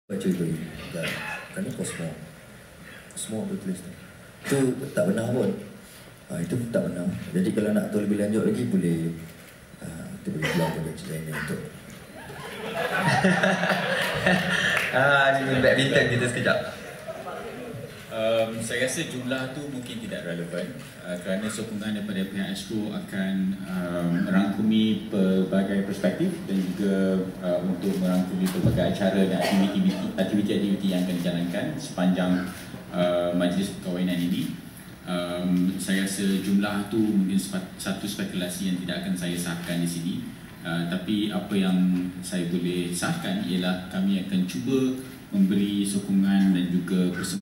Sidang Akhbar Majlis Perkahwinan Mawi 6
Mawi jawab isu tajaan RM5,000,000 oleh ASTRO